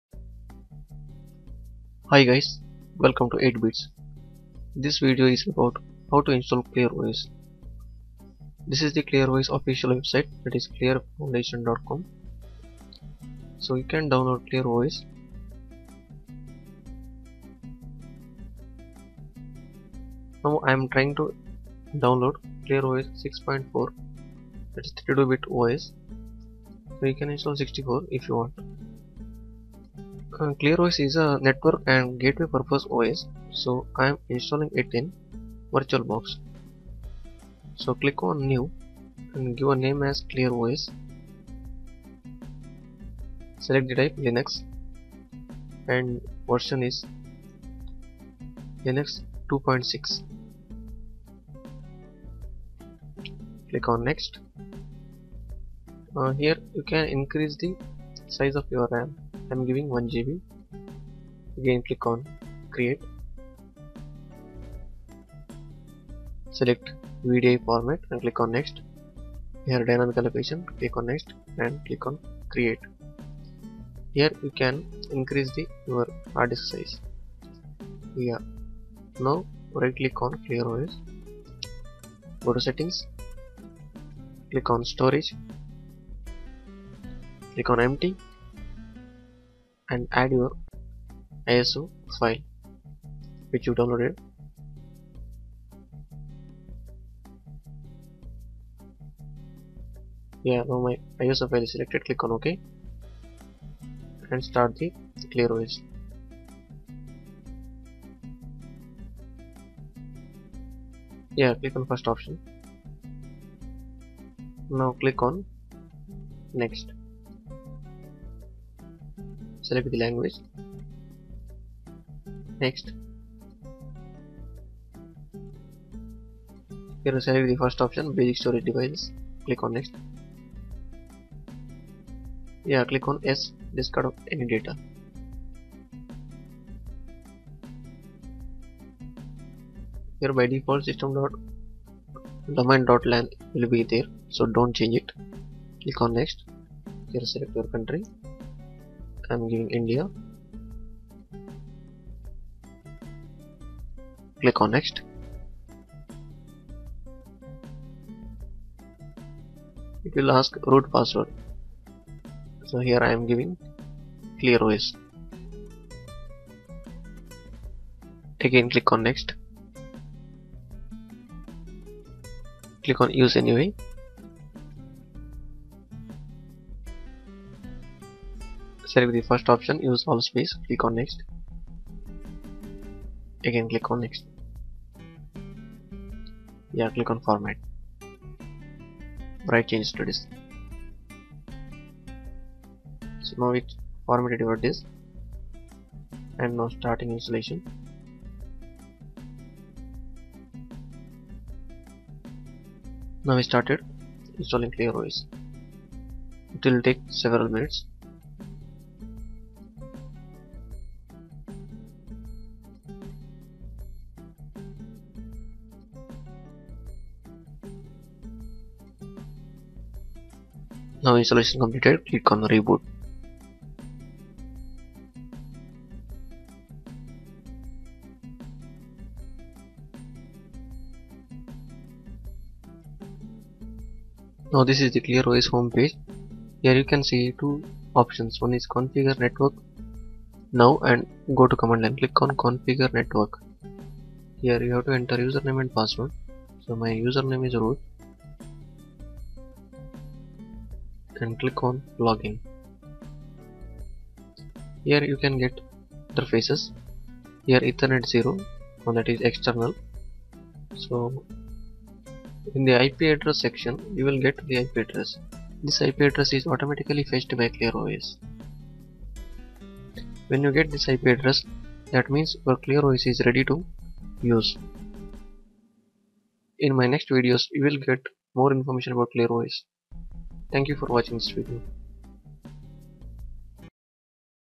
How to install ClearOS 6.4 [2013]
In this video i'm going to show you about how to install 'ClearOS community 6.4'. ClearOS is a next generation Network, Gateway and Cloud based OS. It acts as Firewall Using this OS you can monitor your bandwidth and network traffic. It helps a lot for Linux users.